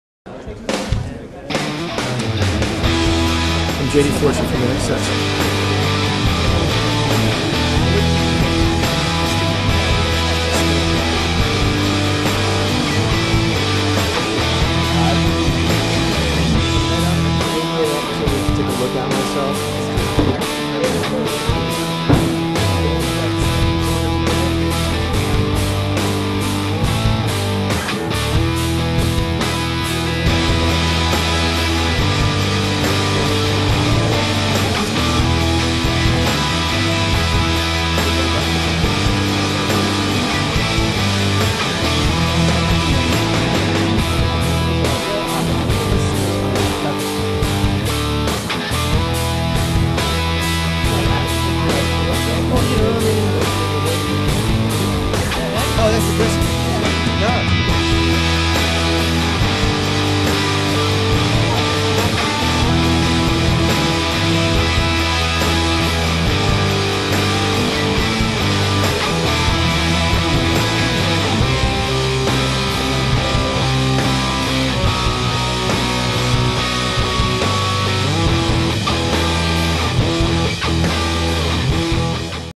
INXS - AUSTRALIA NEWS
Talking to INXS about their search for a new frontman